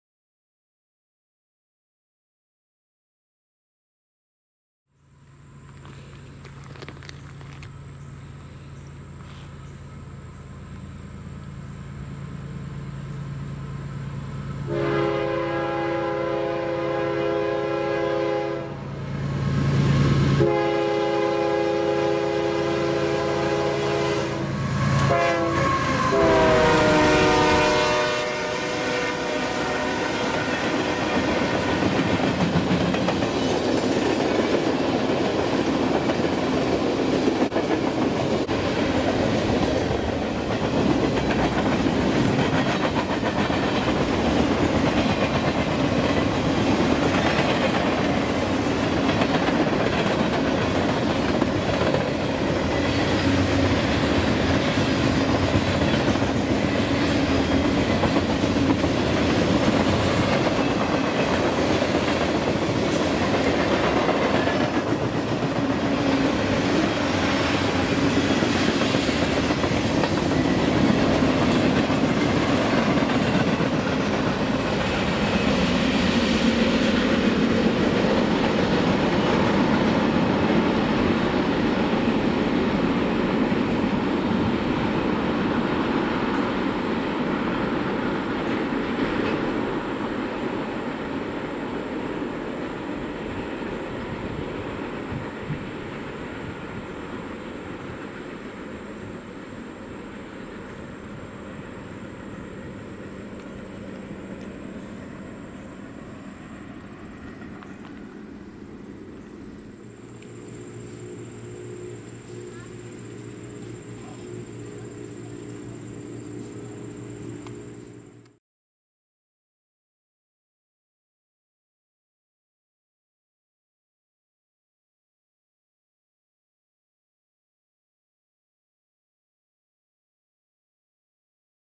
CSX Manifest Southbound At Cherry Hill
A CSX manifest train runs southbound, the conductor gives me a thumbs up as he passes by. There is a boat on the Potomac River and I caught him trying to keep cool  on this hot humid afternoon. My weather shows it’s 90*F and feels like 97*F or 32*C feeling like 36*C !